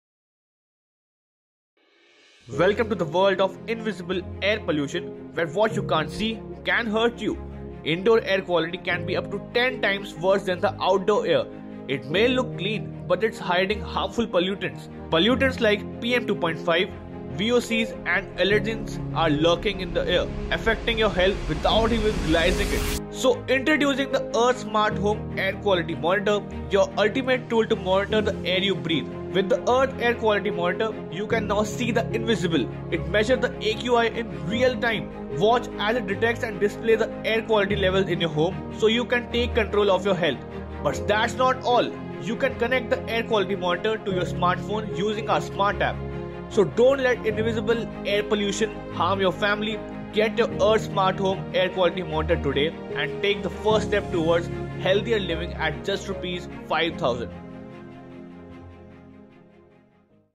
Welcome to the world of invisible air pollution, where unseen pollutants can harm you.
Indoor air quality can be 10 times worse than outdoor air, hiding harmful substances.

Pollutants like PM2.5, VOCs, and allergens are present and affecting health unknowingly.

Introducing the AIRTH Smart Home Air Quality Monitor for real-time AQI monitoring.

Detect and display air quality levels in your home, taking charge of your health.

Connect the Air Quality Monitor to your smartphone via the 'Smart App' for real-time updates.

Prevent invisible air pollution from harming your family with the AIRTH SMART Home Air Quality Monitor.